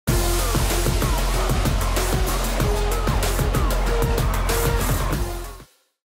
KeepAClearHead with CLEAR. No dandruff`, no doubts.
Always keep a Clear head because no dandruff`, no doubts. Take Safawi's challenge on, show off your skills and stand a chance to win* a trip to watch ElClásico in Spain!

`No visible flakes with regular use.